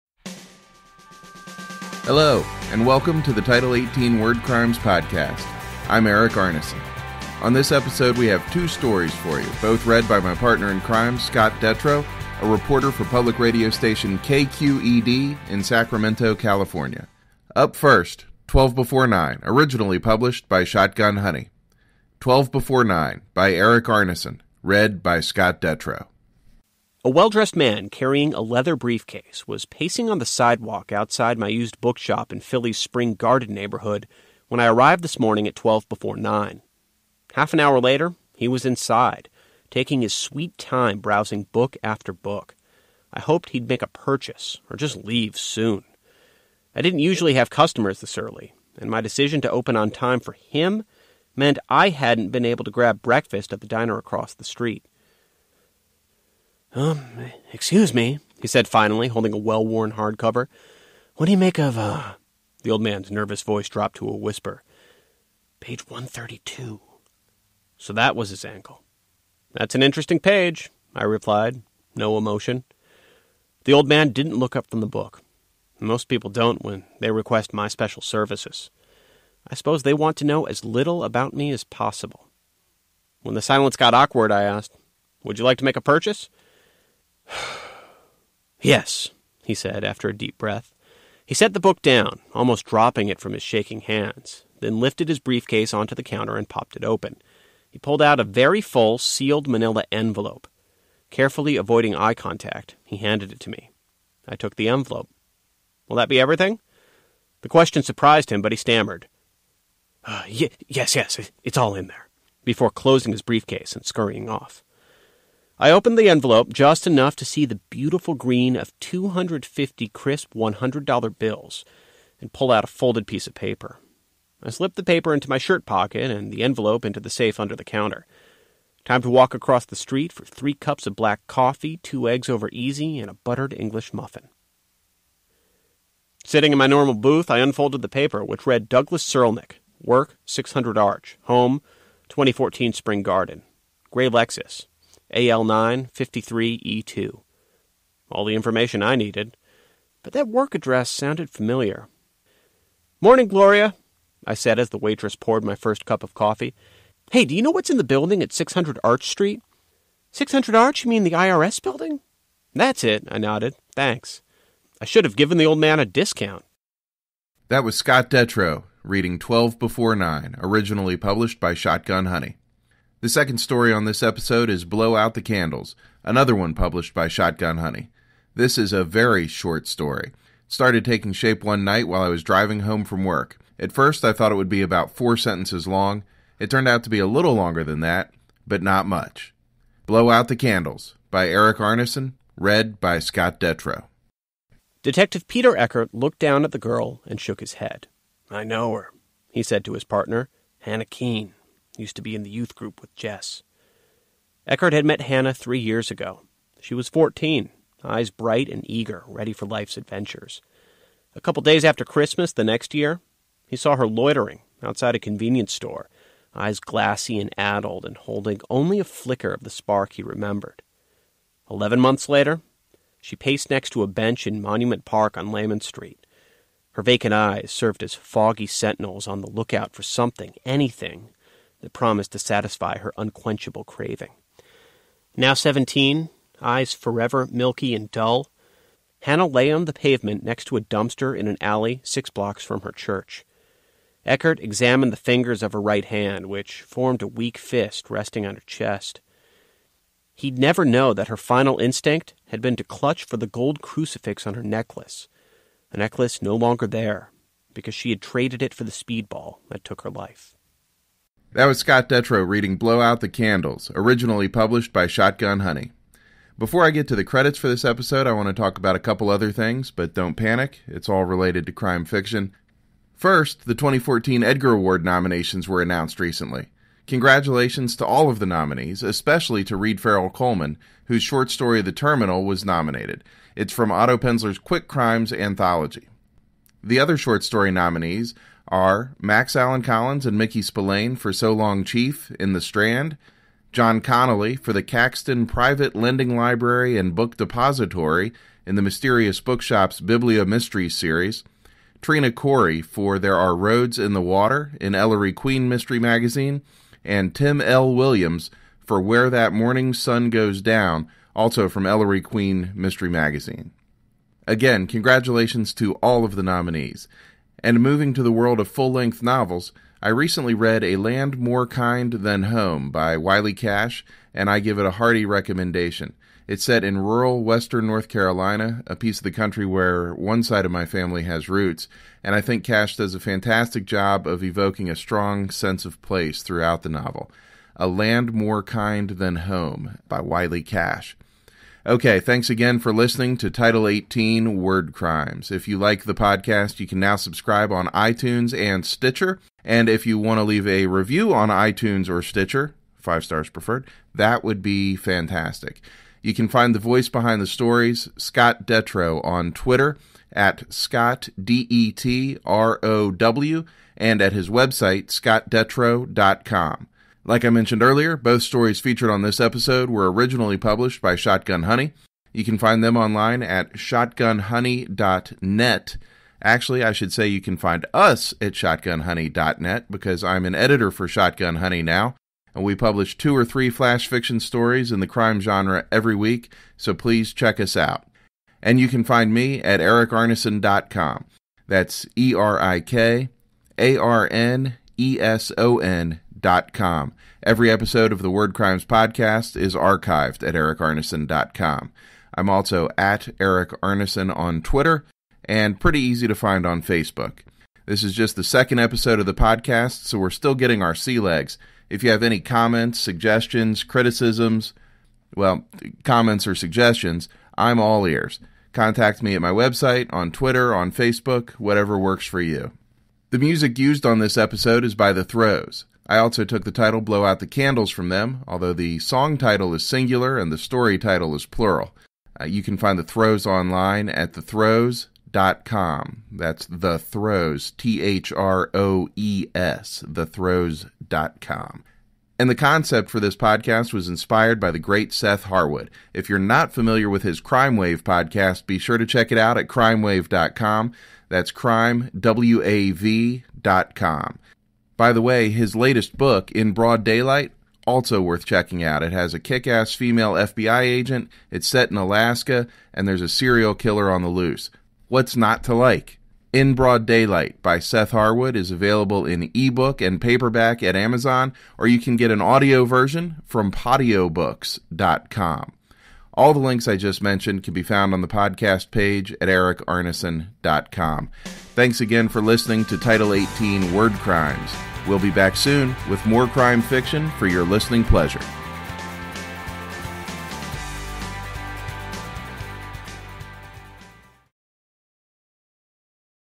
Twelve Before Nine & Blow Out the Candles by Erik Arneson (S1E2)
Erik Arneson and Scott Detrow team up to bring you "Twelve Before Nine" and "Blow Out the Candles," two crime fiction stories originally published by Shotgun Honey.